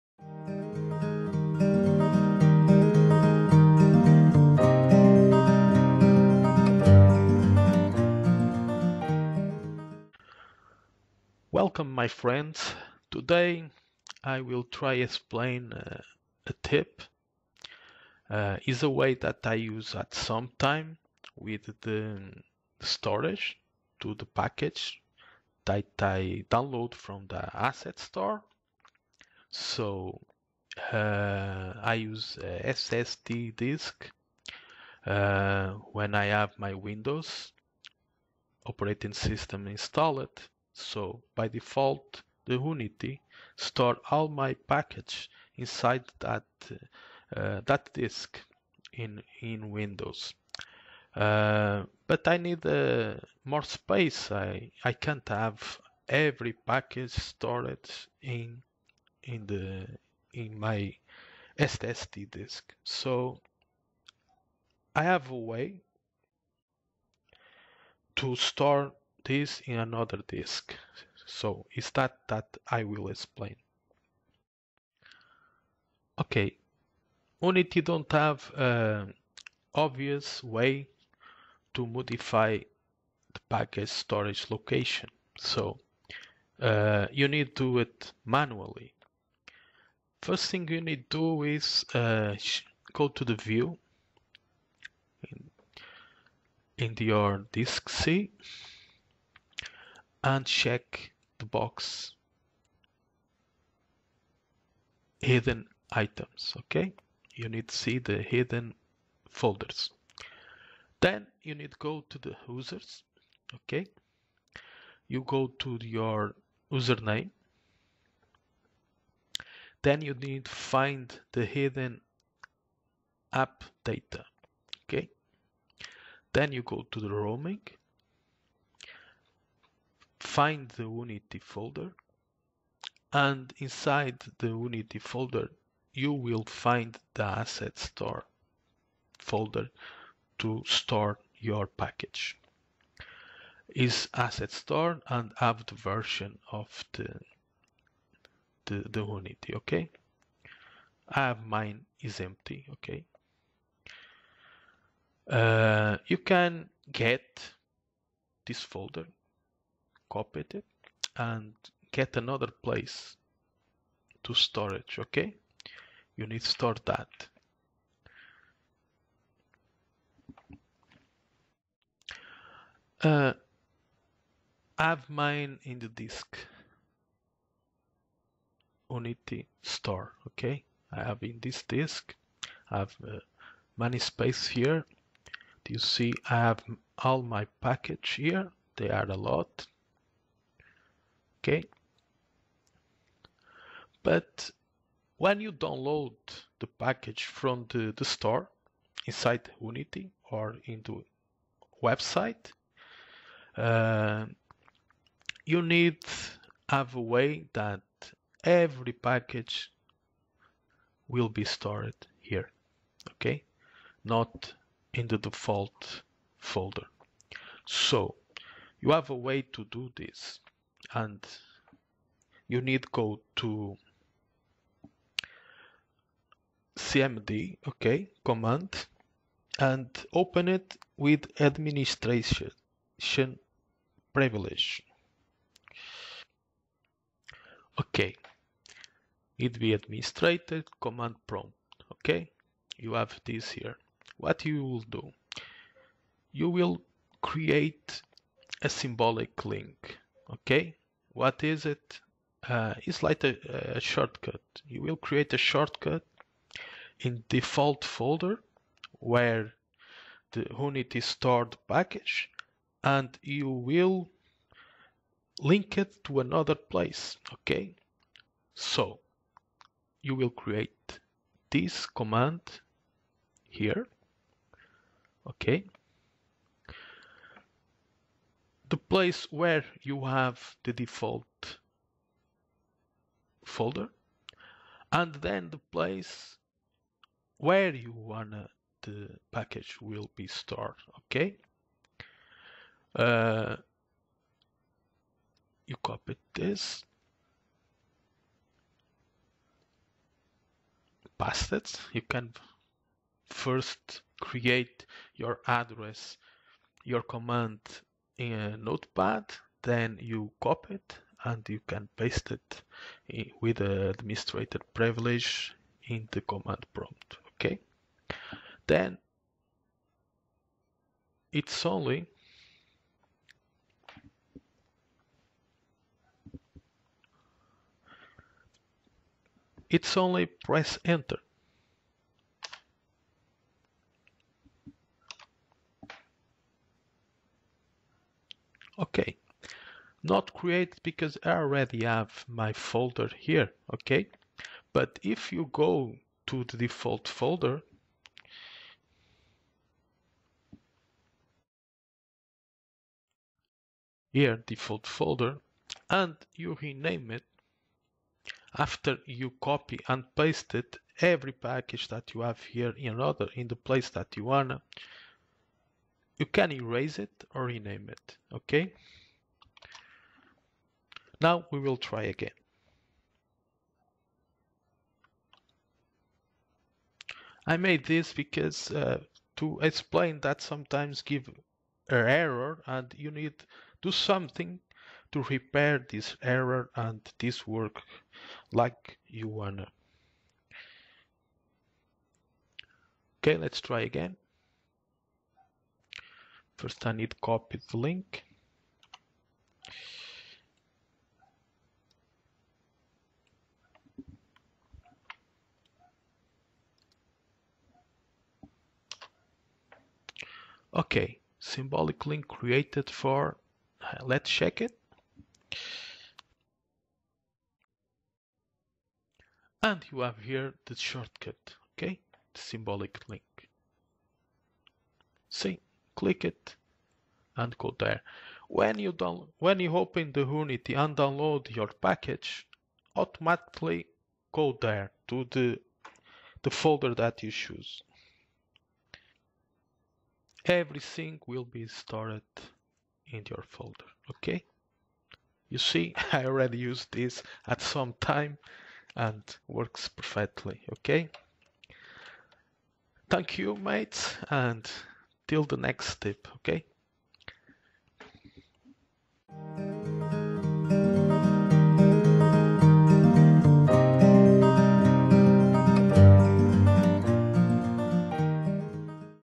Unity [Useful Tips] Relocate the Default Asset Store Folder [Tutorial] 03
Unity - Relocate the Default Asset Store Folder

Unity packages are a handy way of sharing and re-using Unity projects and collections of assets;
Unity Standard Assets and items on the Unity Asset Store are supplied in packages.
By default this packages are located in the hidden folder "C:/Users/(your user name)/AppData(hidden)/Roaming/Unity/Asset Store(version)"
But if the disk where the Windows OS is installed is for example an SSD, it is convenient to have these packages stored on another disk with more capacity.
Because Unity does not have an obvious way to modify the package storage location, this tutorial will explain a simple way to do it in Windows OS.

It will help me with this YouTube channel.

*** Get Unity ***
*** Choose a plan that’s right for you ***
*** Unity Personal ***